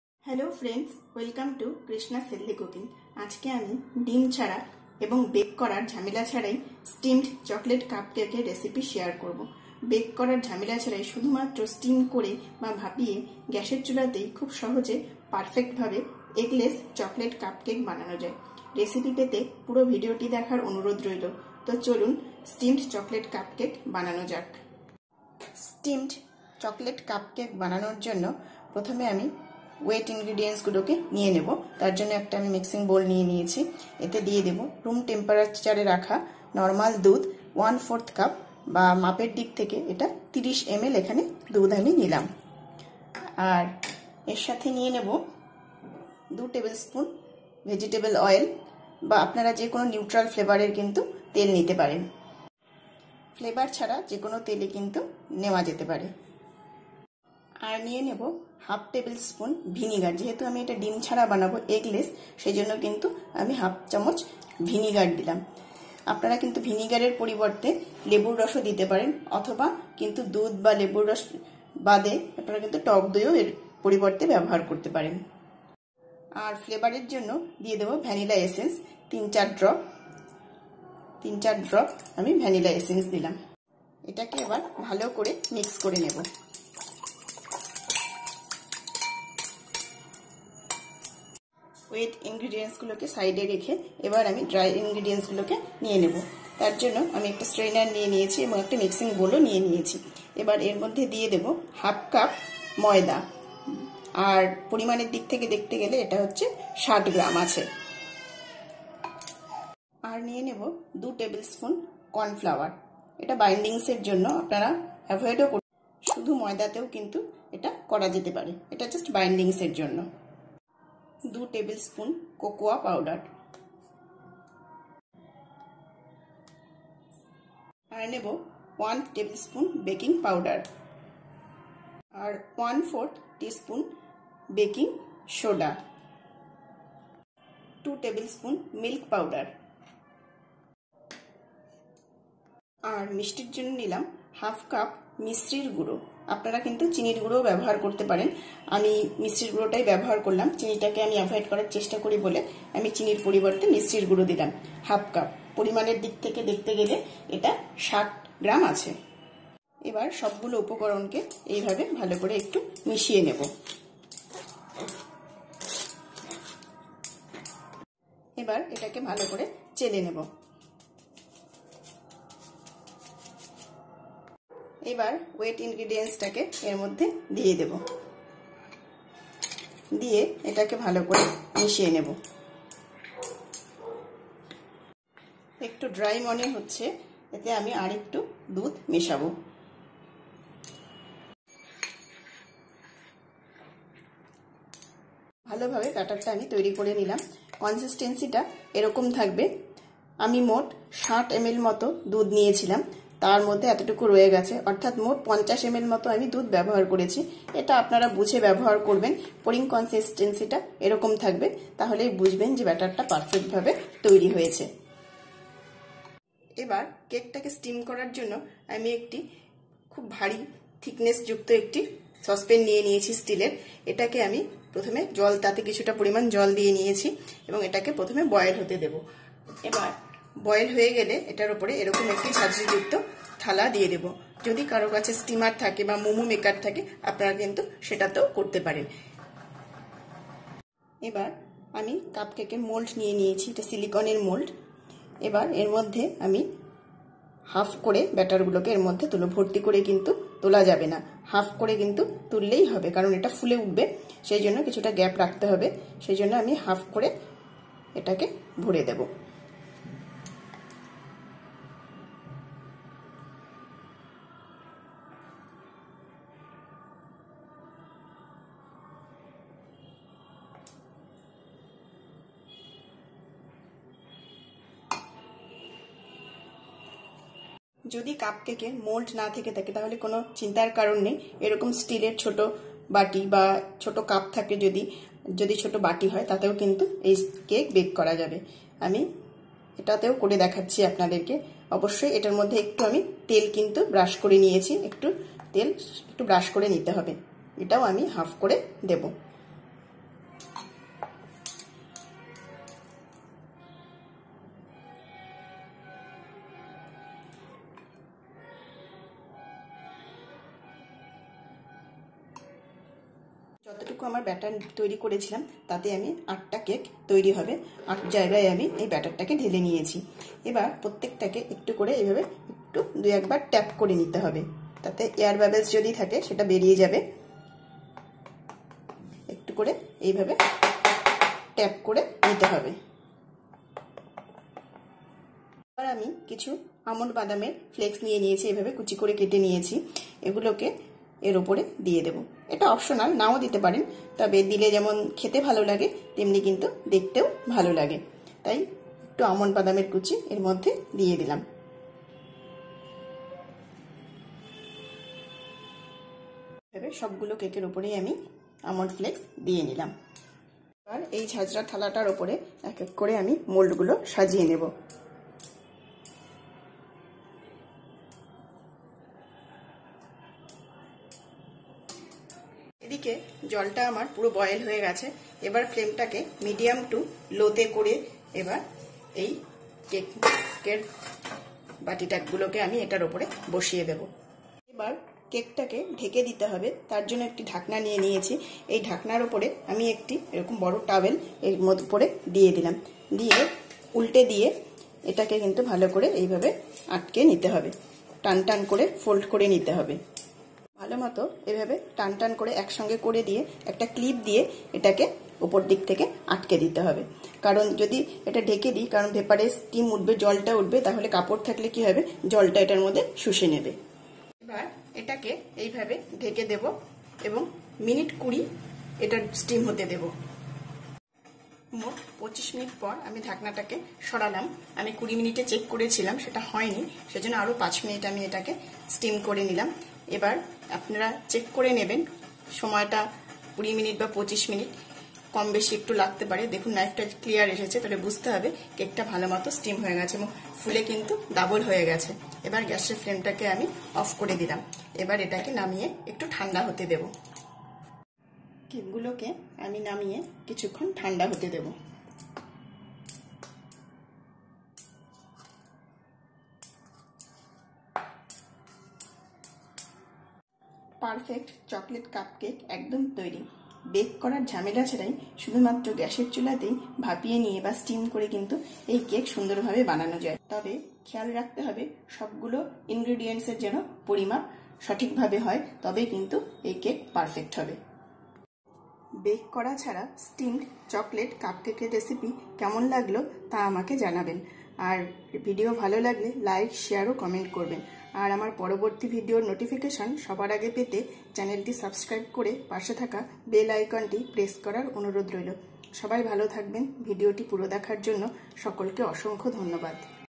Steamed Eggless Chocolate Cupcake (No Bake),বেক করার ঝামেলা ছাড়াই শুধু ভাঁপিয়ে বানান চকোলেট কাপকেক
বেক করার ঝামেলা ছাড়াই শুধু ভাঁপিয়ে গ্যাসের চুলাতে বানিয়ে ফেলুন ডিম ছাড়া চকোলেট কাপকেক,, খুবই সহজে সঠিক পরিমাপ মত সব উপকরণ নিলে পারফেক্ট ডিমছাড়া চকোলেট কাপকেক বানানো যায়।